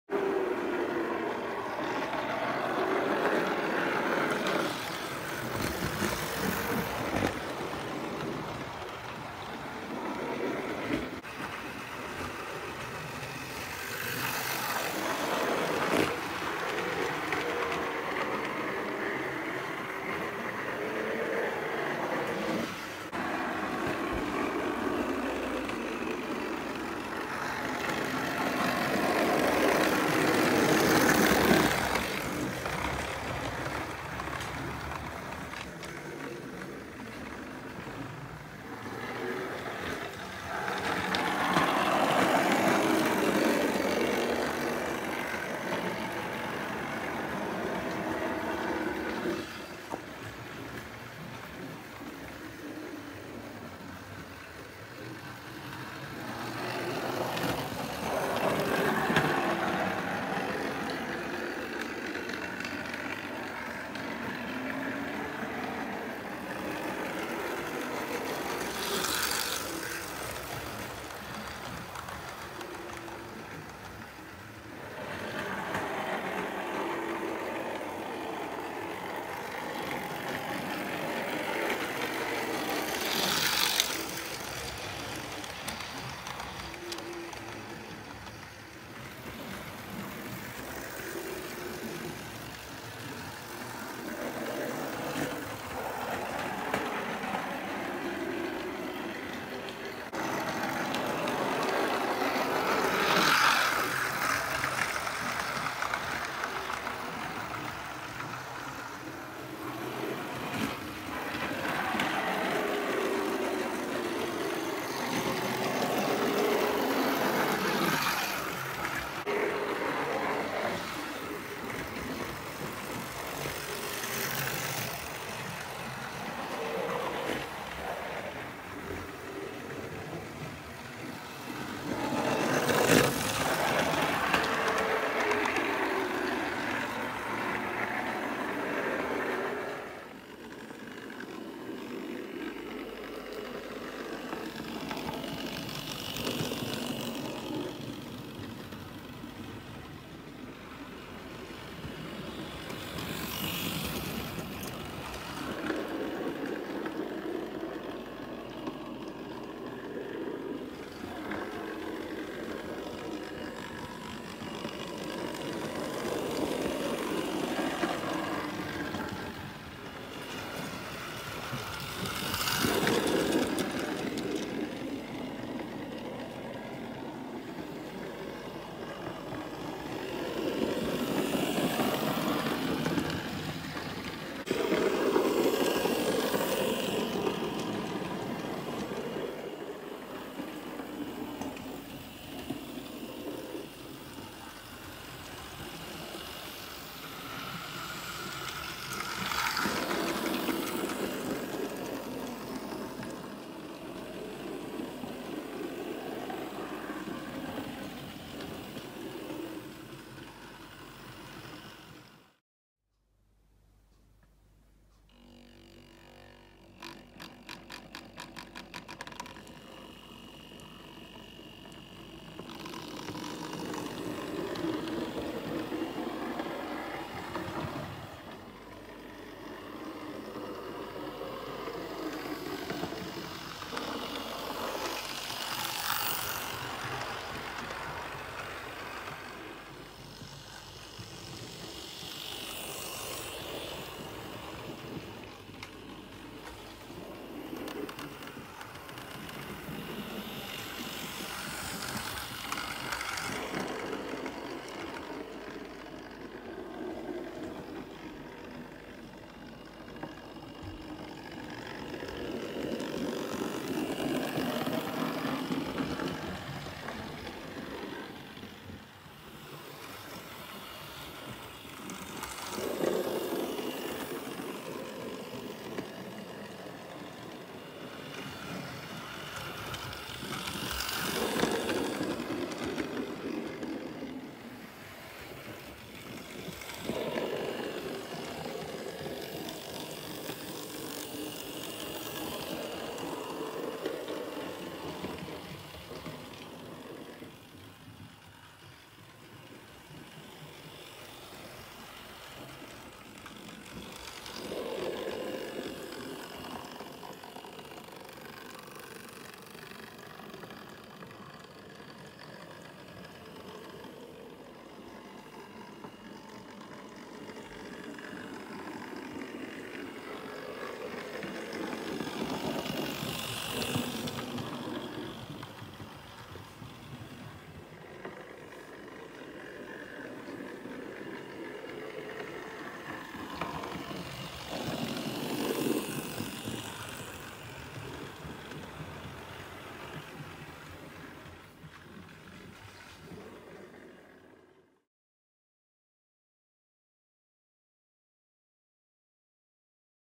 Hornby Dublo Running Session | 00 gauge model railway
This weeks video is of a running session I have composed running my two Hornby Dublo locomotives, Class 20 and Standard 4mt tank. Great heavy locos that can pull tons more than demonstrated in this video. The class 20 hauls a passenger train consisting of modern rolling stock mixing old and new coupling. The standard 4 hauls Hornby Dublo MK1’s and freight. It is believed that this Dublo 4mt came with the freight in a set back in its day.

I have a discord server where you can talk to like minded people and you are most welcome to join!